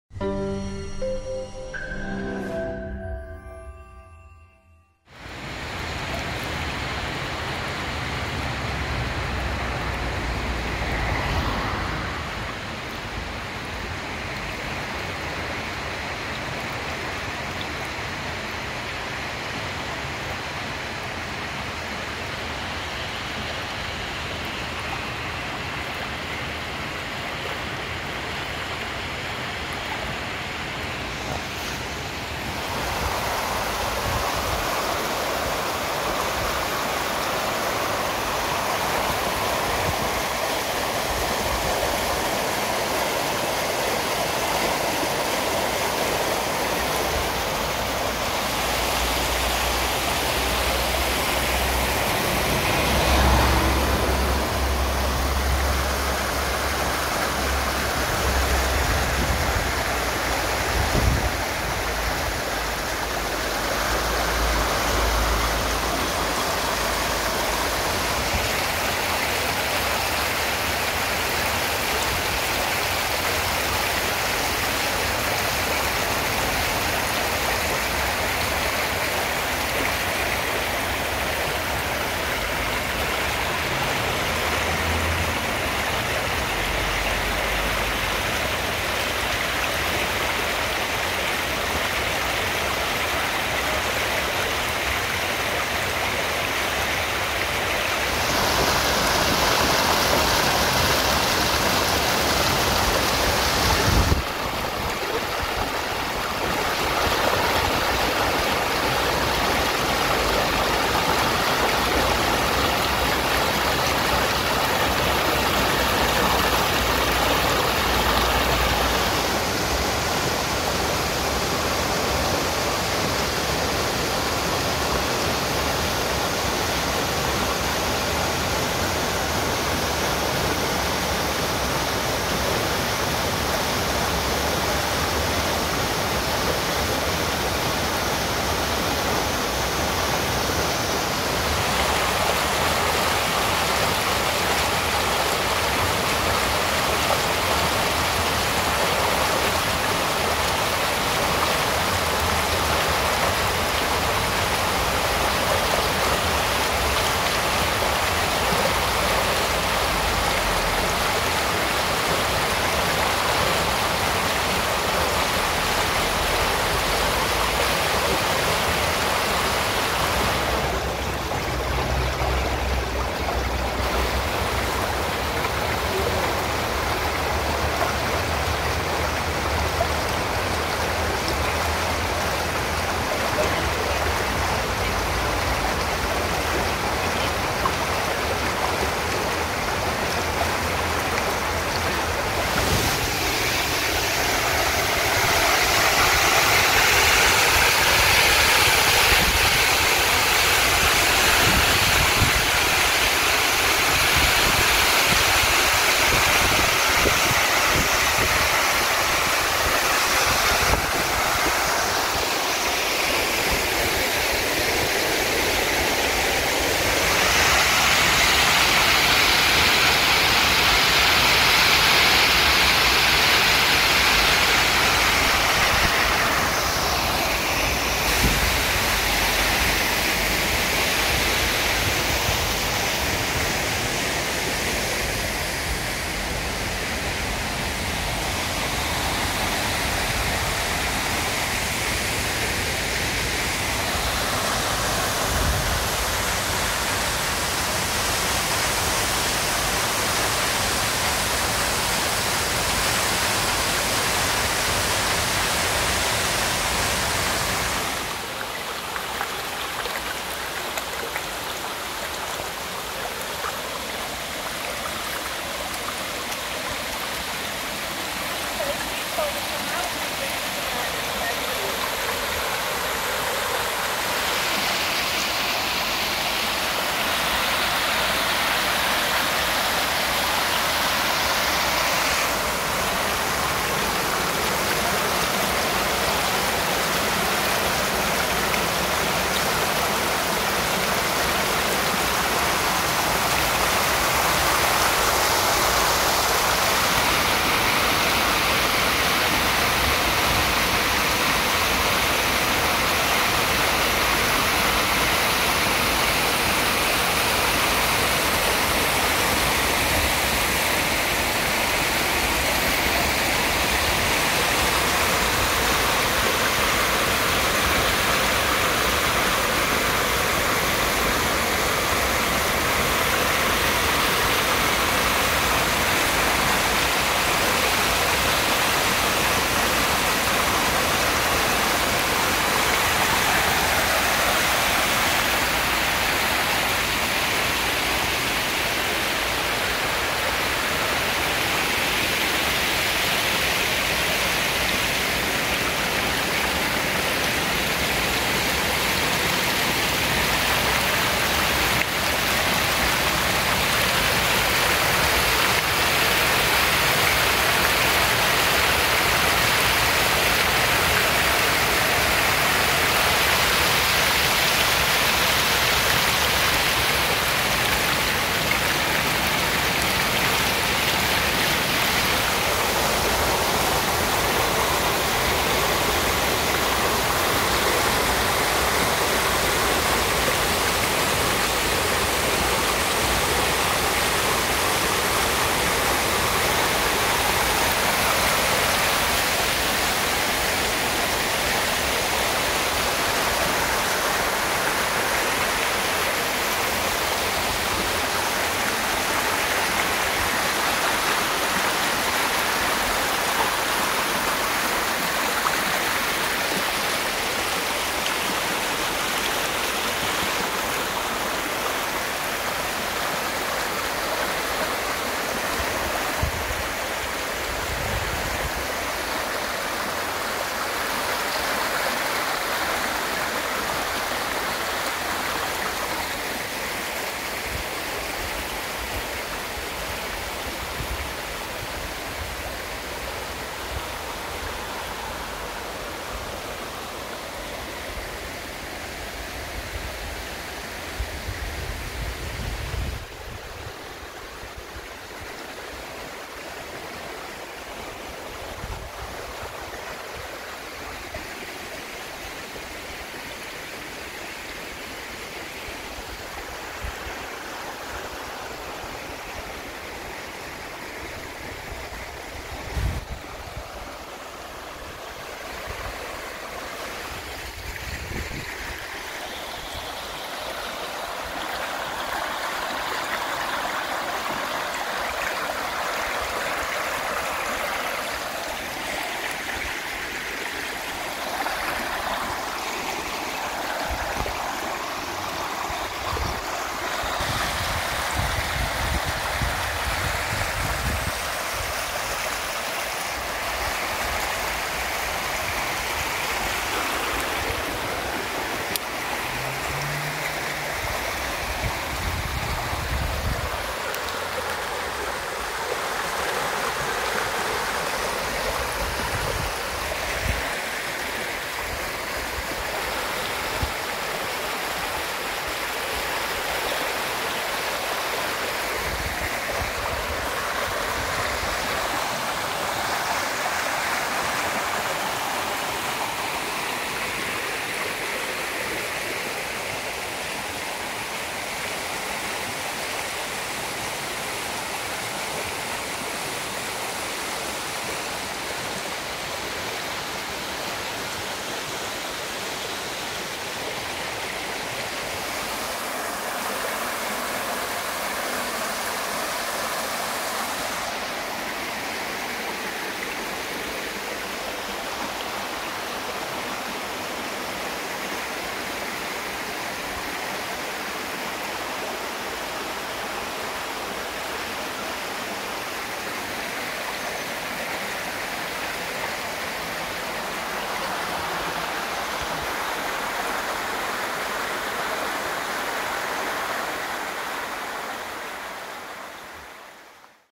North Wales: Rhaeadr Dyserth Waterfalls - July 2019 - 4K Ultra-HD Video Footage
This video is made up from short clips I captured while exploring Dyserth Waterfall and the surrounding area. The clips are from Tuesday 2nd July 2019 when I visited the location for a few hours during a family holiday in north wales.

Footage starts from the car park bridge, beside the road, close to the falls entrance and cafe.
We followed the falls through the woodland and upstream as far as we could safely get to.
My favourite part is probably where the footpath through the woodland crosses the water. The holes in the concrete path are blocked meaning the water now flows over the top, which made for some great footage and pictures.

All clips are captured in 4K Ultra-HD at 30fps using a Samsung Galaxy S8 smartphone.